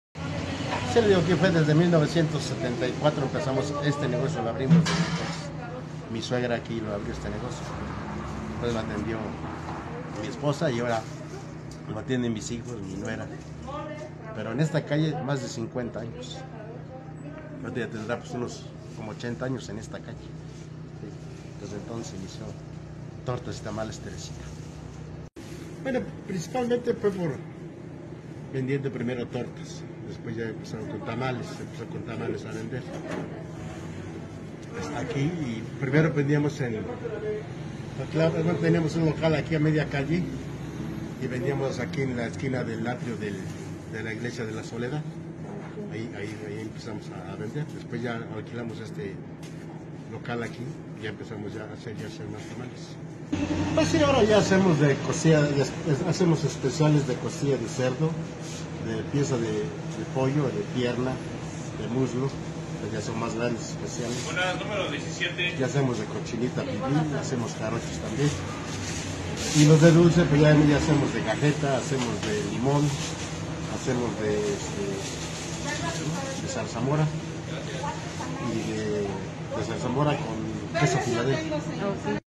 Tortas y tamales Teresita fue fundado en 1974 desde entonces han innovado en el sabor de sus tamales
"Tortas y tamales Teresita" fue fundado en 1974, desde entonces han innovado en el sabor de sus tamales. Actualmente ofrecen especiales de costilla de cerdo, pierna de pollo, cochinita pibil, limón y cajeta.